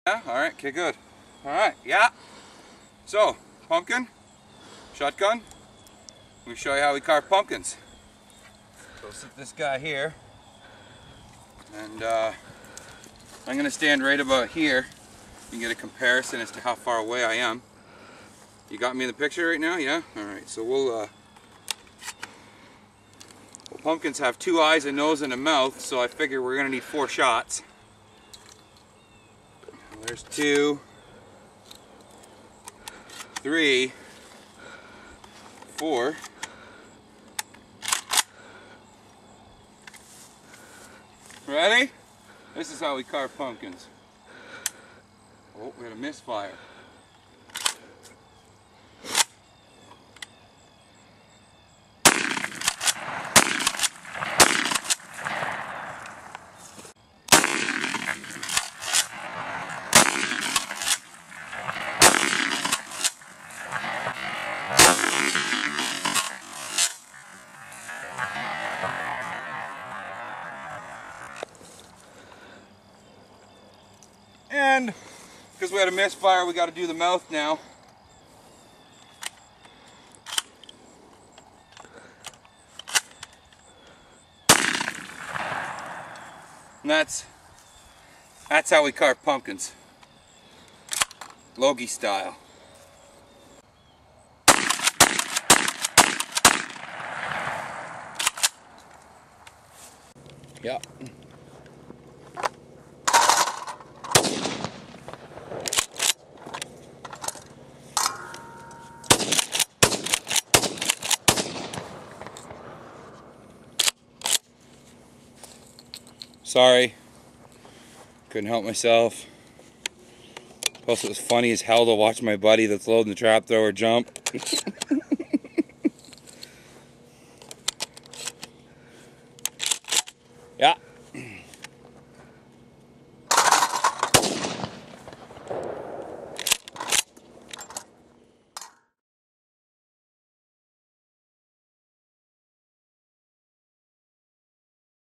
Carving Pumpkins with a 12 Gauge Pump Shotgun
so I think the end result turned out well don't you ?